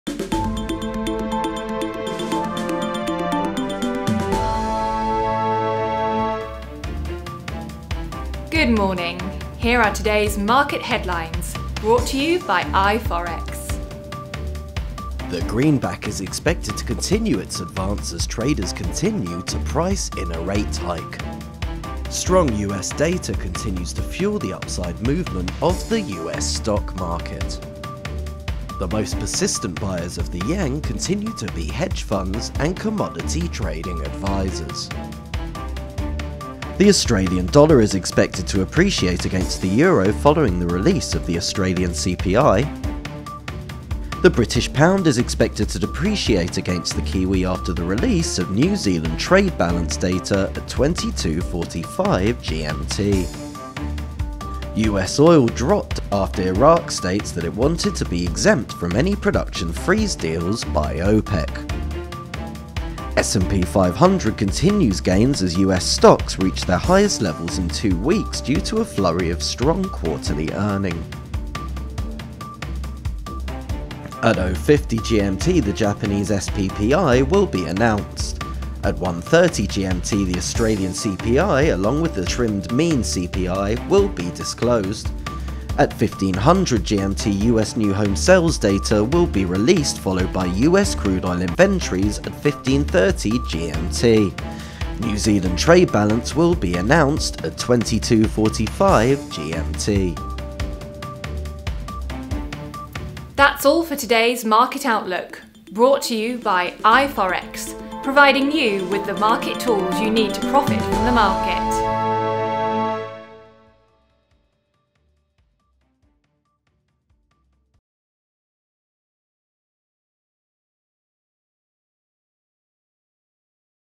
iFOREX Market Headlines 26-10-2016: USA Economy, US Stocks and JPY.
Capital is at risk

- Dollar continues gains amid Fed rate hike expectations.

- Equity markets continue heading higher as economic data
  supports the US economy.

- Hedge funds and commodity advisers remain bullish on the yen.